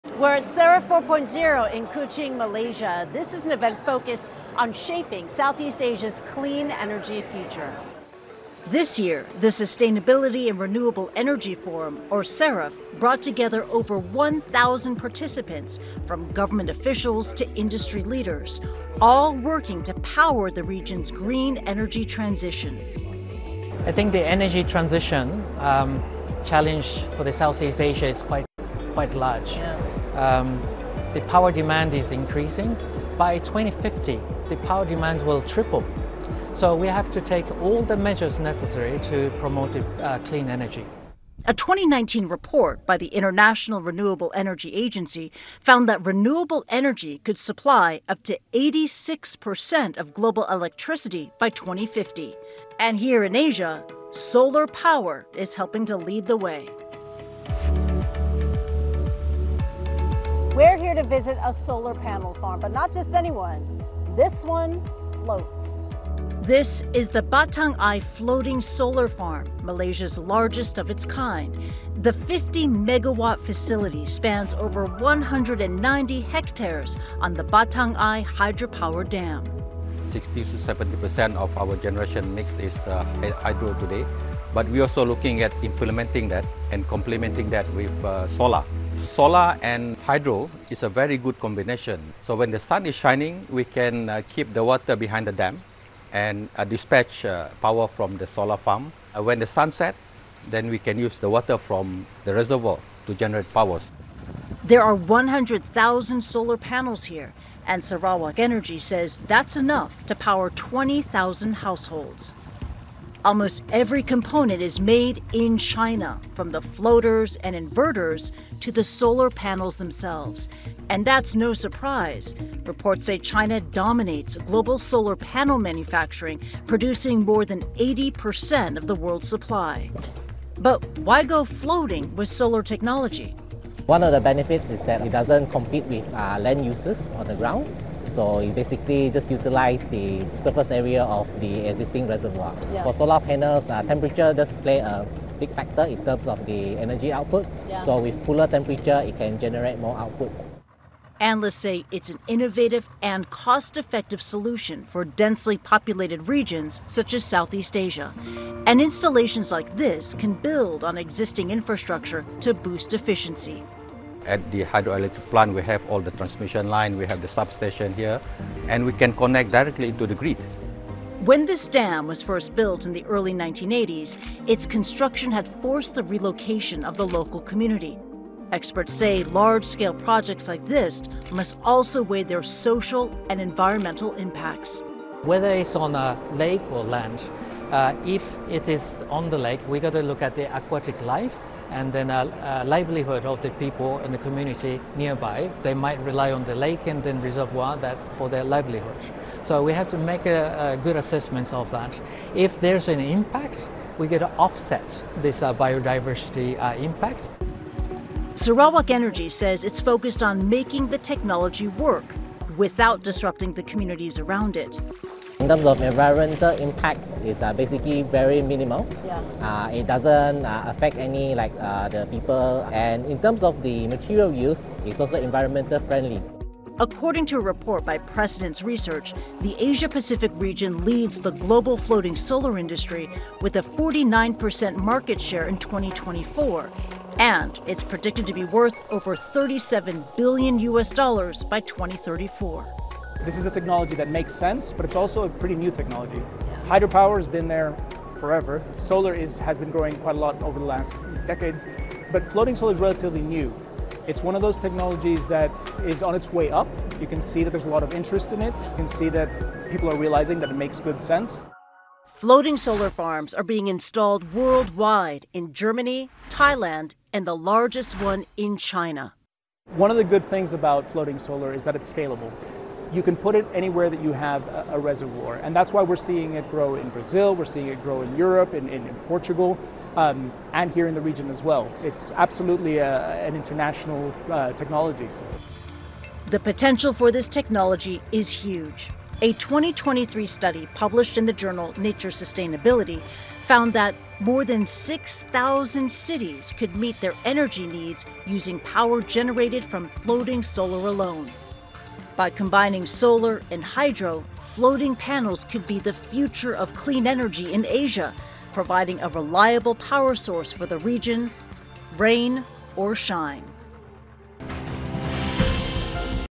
Floating solar power: Asia's game-changer in clean energy
The Asia-Pacific region, the world’s biggest energy consumer, is leading a clean energy revolution through innovations like floating solar farms. With regional power demand expected to triple by 2050, major projects such as the Batang Ai floating solar farm -- Malaysia’s largest -- are mixing solar with hydropower to supply reliable, scalable electricity for tens of thousands of homes, most of it made with China’s globally dominant solar components. Experts say floating solar’s flexibility means it can be installed anywhere with a reservoir, and the region already holds nearly half the global market share. Global installations are rising, with the sector predicted to be worth $37 billion by 2034 and Asia primed to be at the forefront. For Marketplace Asia, CNN's Kristie Lu Stout reports from Sarawak, Malaysia -- host of SAREF 4.0 or the Sustainability and Renewable Energy Forum.